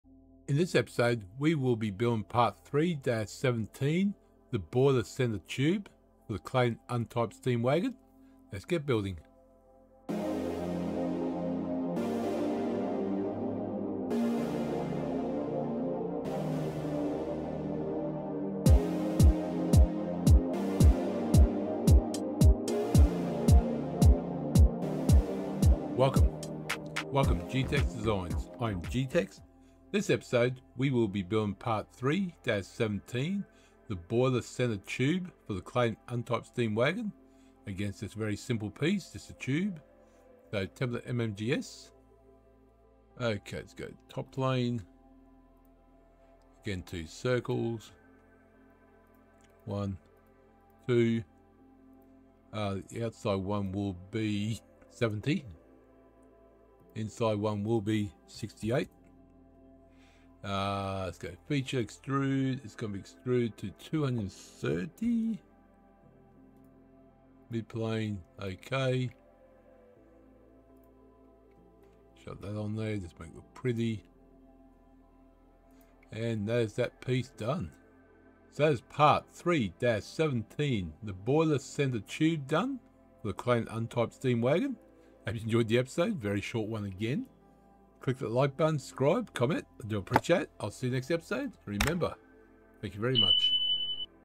E 141 - Part 3-17 - Boiler Center Tube - Clayton Undertype Steam Wagon
Let's Build the Clayton Undertype Steam Wagon, this episode we will build Part 3-17, the Boiler Center Tube.